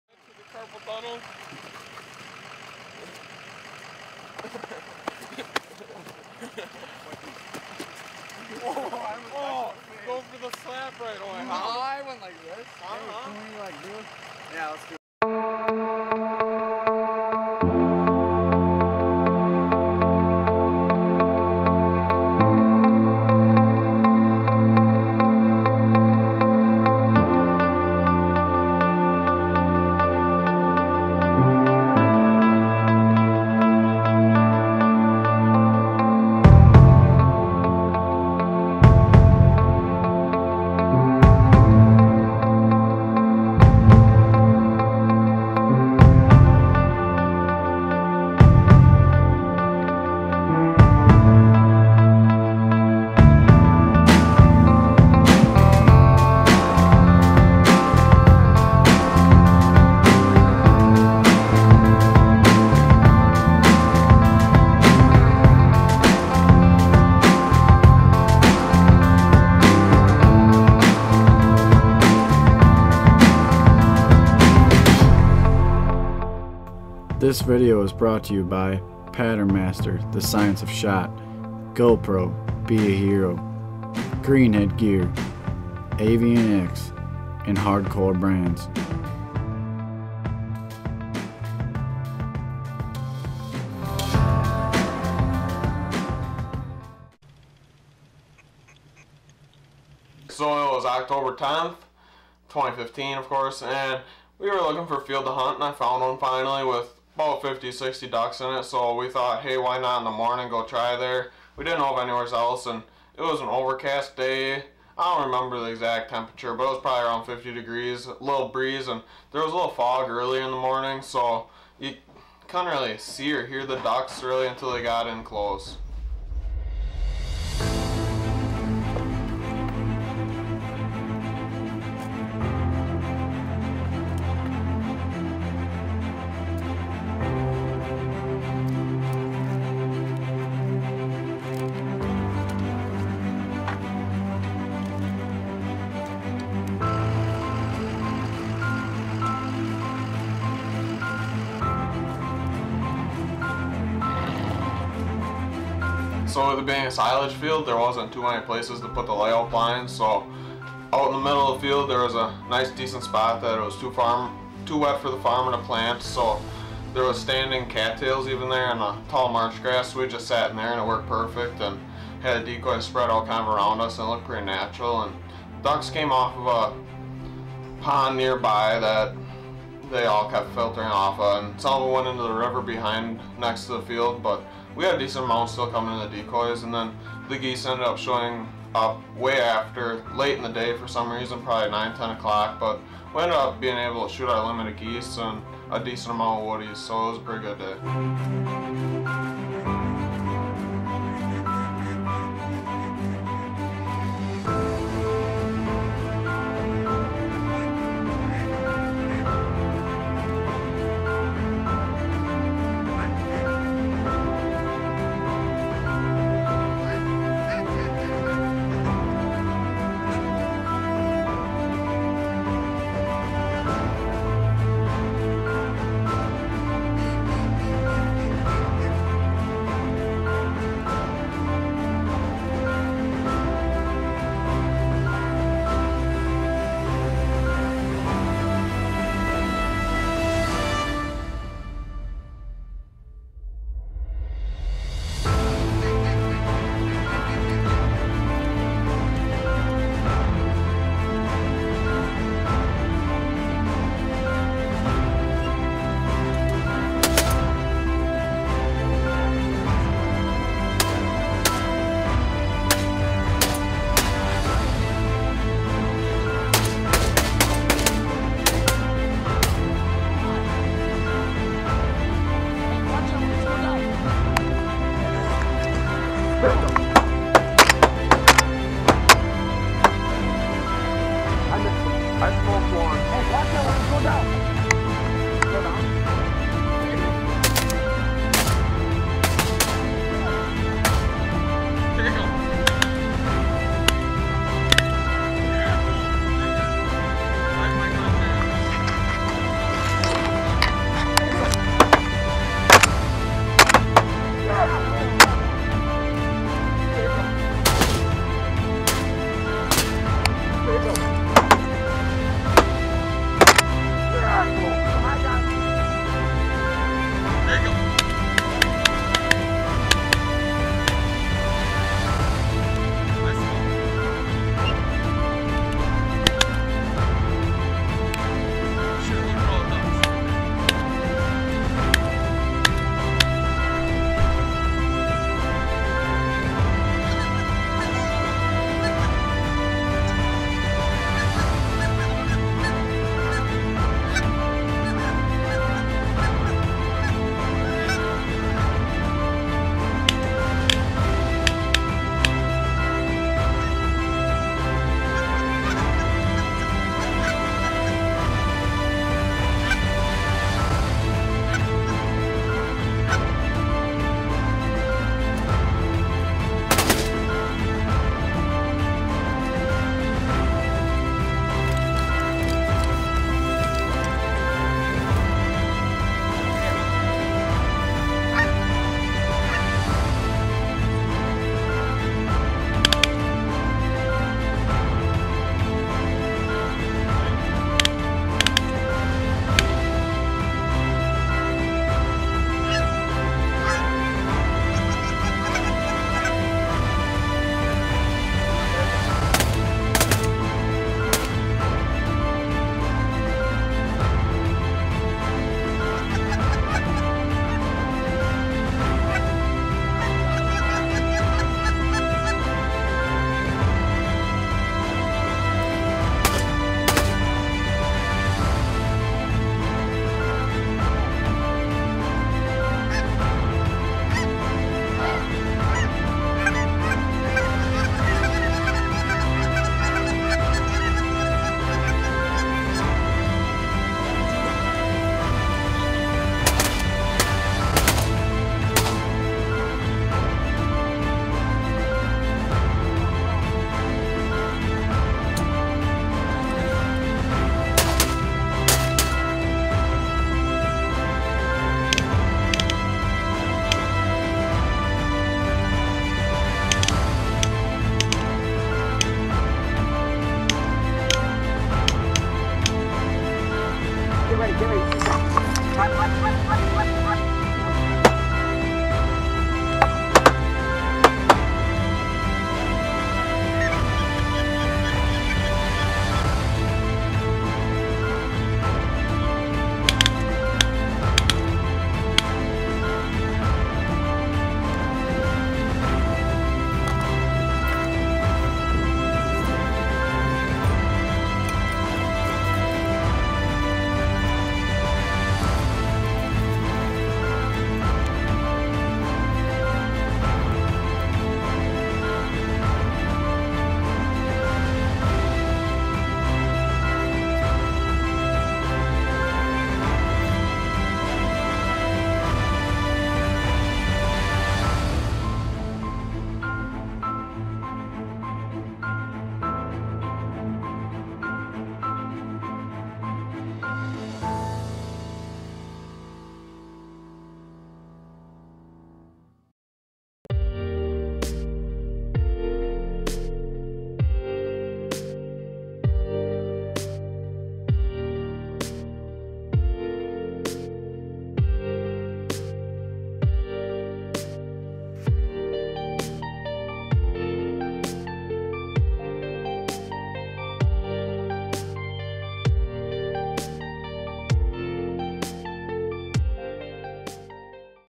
Duck and Goose Hunting: TGD Outdoors Season 3 Ep 3 "Circles"
This is the second to last episode for season three here at TGD Outdoors. On this hunt, we take a trip to hunt some ducks and honkers in a field and it's one you won't want to miss!